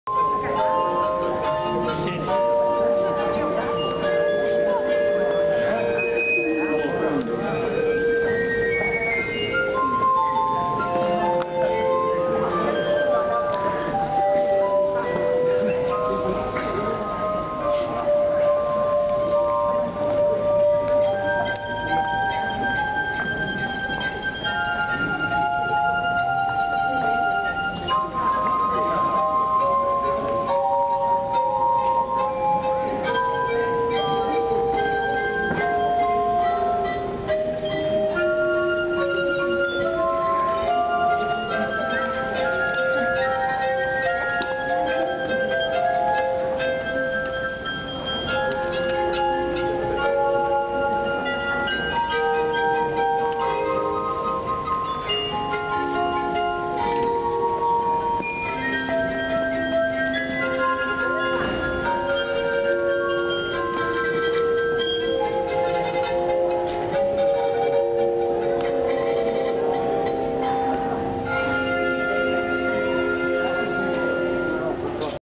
Venice Carneval 2014 2/3
Venezia, Calle Bastion Dorsoduro, davanti alla Chiesa di San Gregorio.
Nastup muzičke trupe u sklopu otvaranja Karnevala 2014.